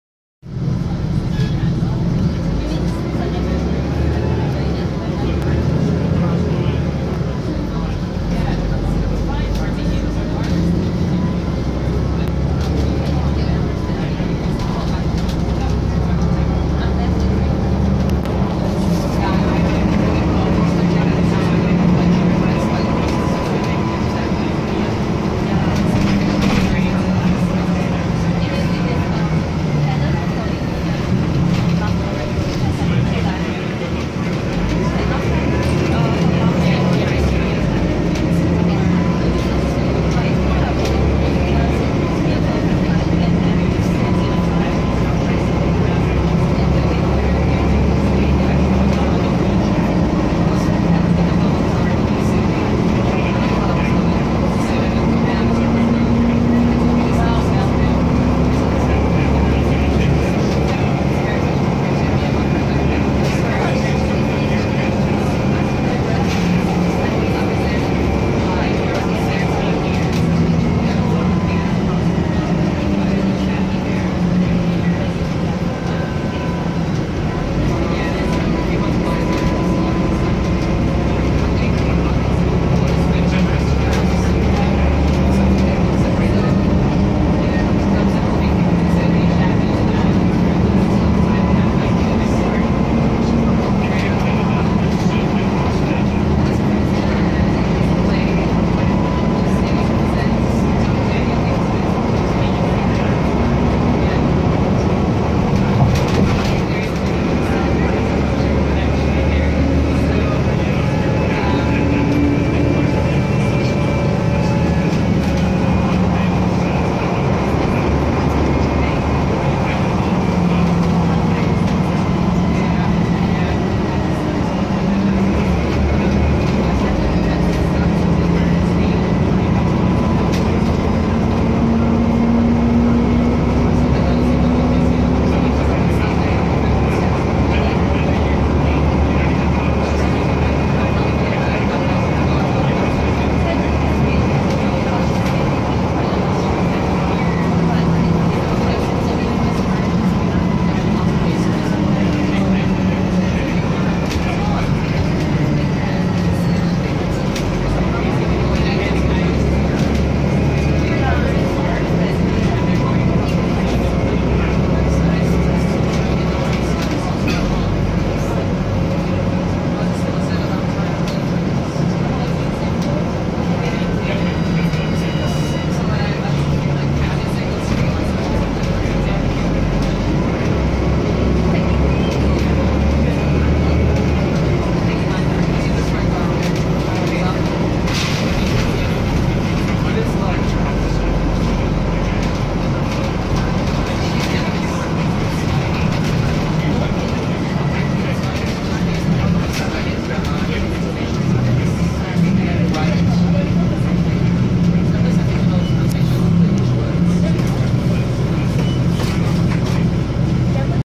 Transperth Bus No. 3017 (Video 1) (1080HD)
Make/Model: Volvo B12BLEA

Body: Volgren "Futurebus"

Engine: Volvo D12E340, 340HP inline 6 cylinder, Euro 5, diesel with exhaust brake

Gearbox: Voith DIWA 864.5

Operator: PATH Transit

Route: 34F